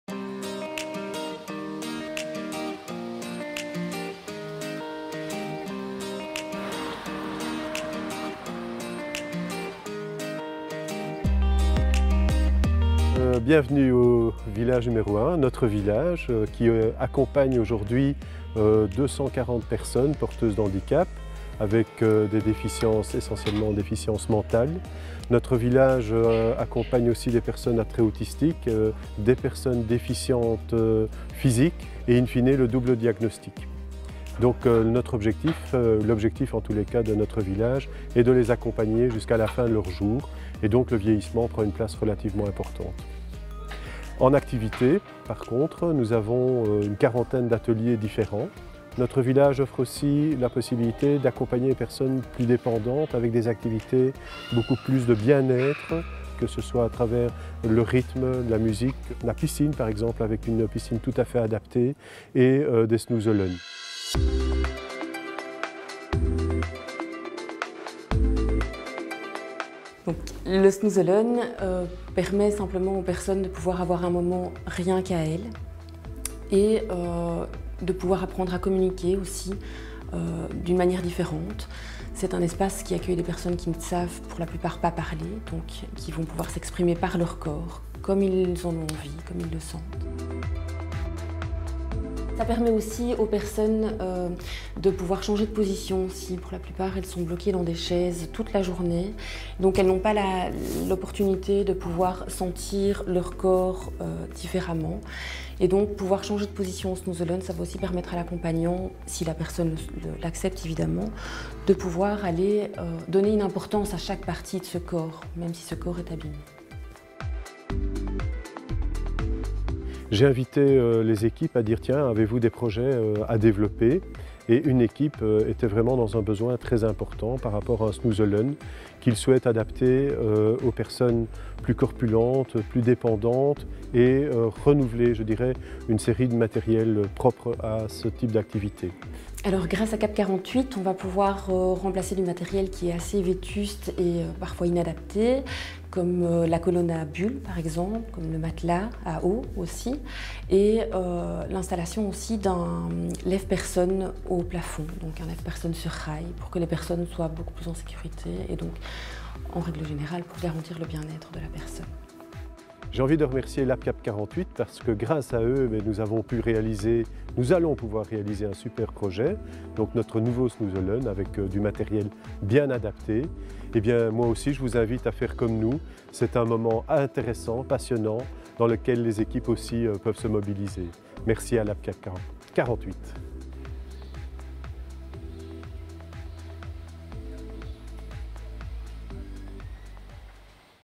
LabCAP48 with CBC | asbl Village n°1
L'association Village N°1, à Ophain, a participé au LabCAP48 pour l'aménagement de leur salle de snoezelen! Grâce à l'argent obtenu, le matériel sera renouvelé et adapté afin de maximiser la sécurité et le bien-être des personnes en grande dépendance.